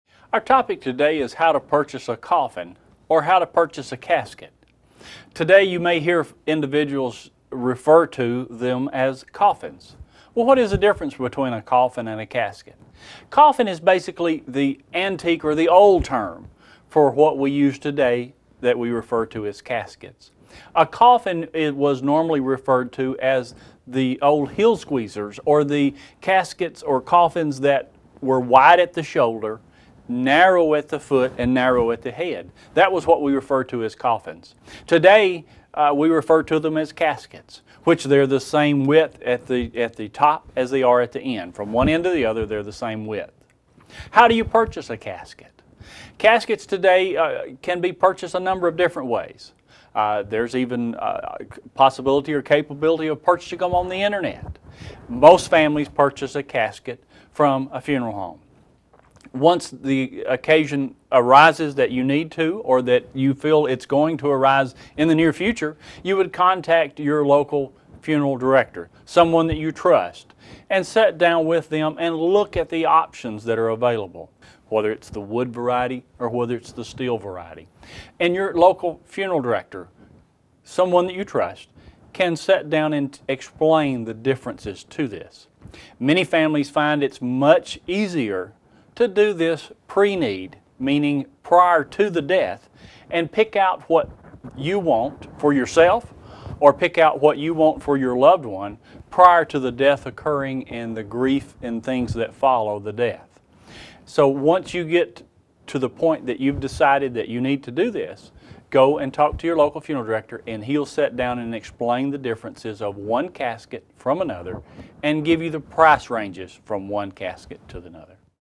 Funeral Planning : How to Buy a Coffin
A coffin, or casket, can be purchased at a funeral home with the help of a funeral director. Buy a coffin prior to death with help from a licensed funeral director and embalmer in this free video on funeral planning.

Bio: Steve Spann is the president of John A. Gupton College, which provides a professional curriculum in the funeral arts and sciences.